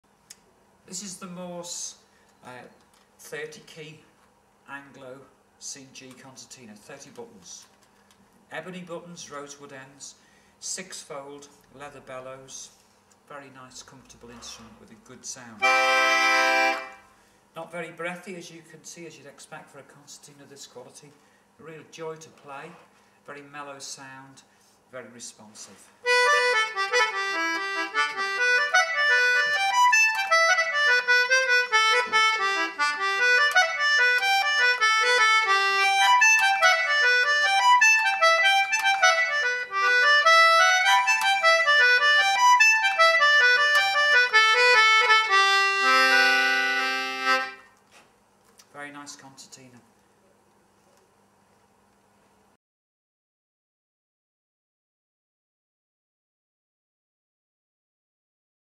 Gina Le Faux plays a Morse 30 Key Anglo Concertina
Gina Le Faux demonstrates  a Morse 30 Key Anglo Concertina at The Music Room, Cleckheaton.